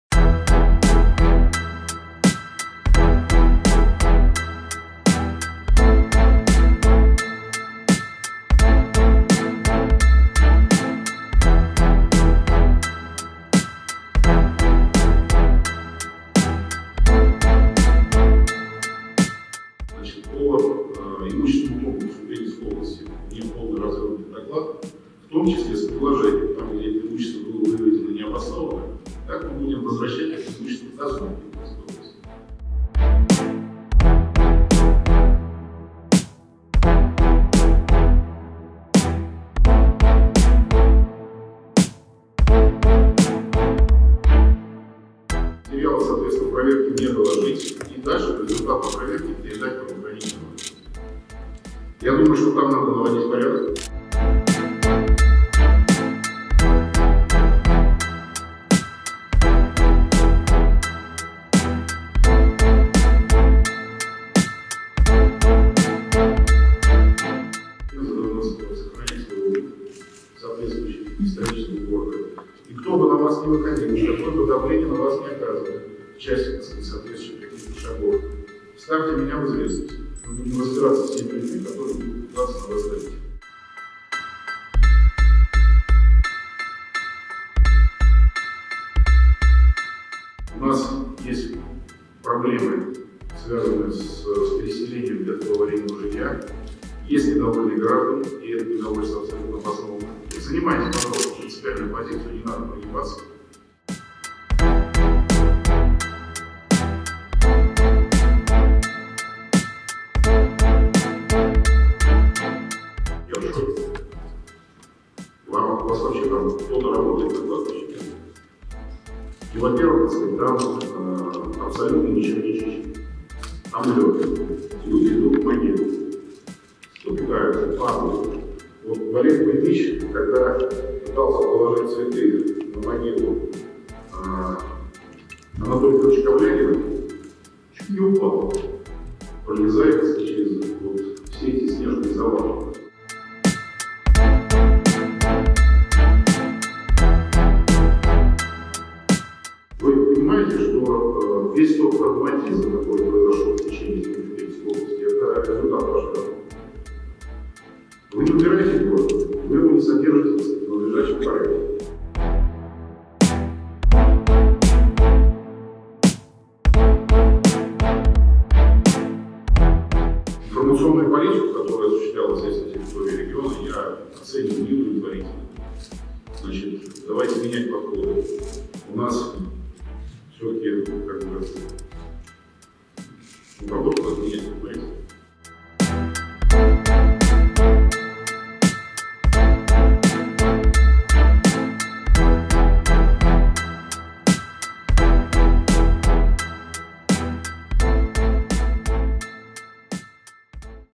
Жесткие перемены в пензенском правительстве после ареста Белозерцева
«Занимайте, пожалуйста, принципиальную позицию, не надо прогибаться», — врио пензенского губернатора Олег Мельниченко провел первую рабочую планерку после ареста Ивана Белозерцева.

Мэра Пензы Андрея Лузгина отчитали за нечищеные улицы и ужасное состояние кладбищ. Также Мельниченко намерен вернуть в собственность региона имущество, которое исчезло при прежнем губернаторе.

Кто попрощался со своими должностями и какие еще изменения намечаются в Пензенской области — в нашем ролике

Волга. МБХ медиа

Сибирь. МБХ медиа

Северо-Запад. МБХ медиа

Урал. МБХ медиа

Юг. МБХ медиа